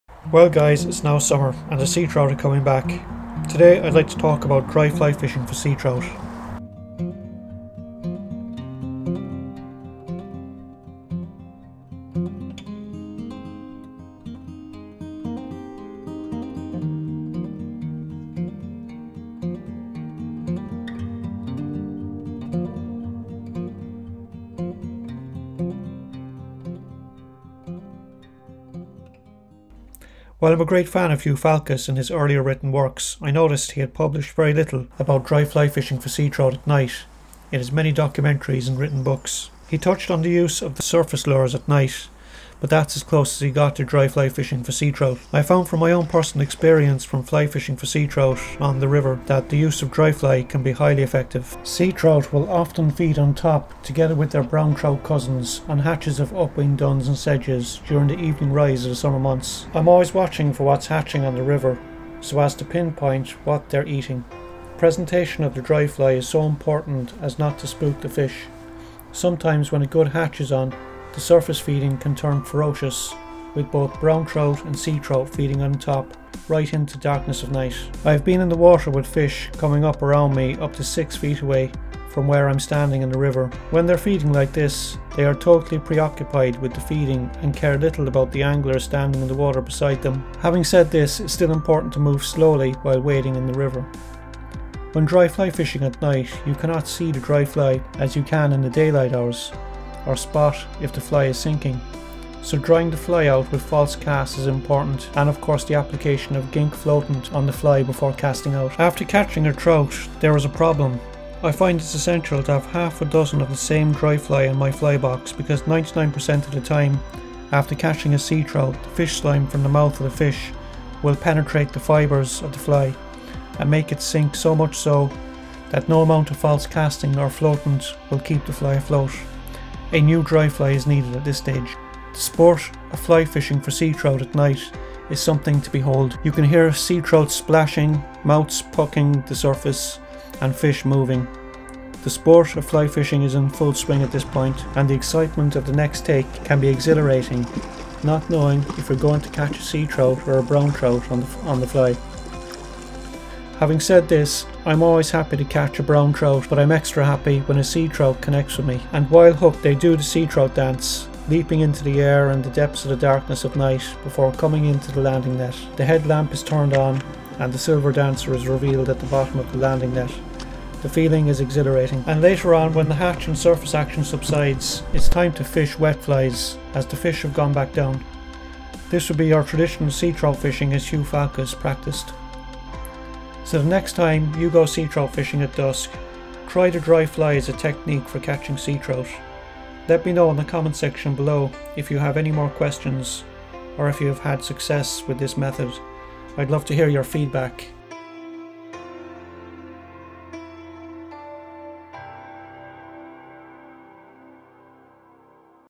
Sea trout on the dry fly
Many anglers believe the only way to target sea trout is by going out in the darkness of night and casting wet flys. However, few know that sea trout can actually be caught in day light hours using the dry fly!

It shouldn't be so surprising since genetically sea trout and brown trout are pretty much the same. A sea trout is simply a brown trout that has travelled to salt water to feed and returns to freshwater to spawn. On their return to freshwater they feed alongside their brown trout cousins on whatever is hatching on the river at the time (upwinged olives, sedges, mayfly).

I discovered myself, whilst targeting brown trout on the dry fly that sea trout would also take a dry fly just as easily, given the opportunity. The sea trout are thinly spread out along my local river and travel in small pods so it takes a bit of luck in finding them. Putting a dry fly over a sea trout and watching that fly get plucked from the surface by a sea trout and tightening into that fish is top sport for the game angler.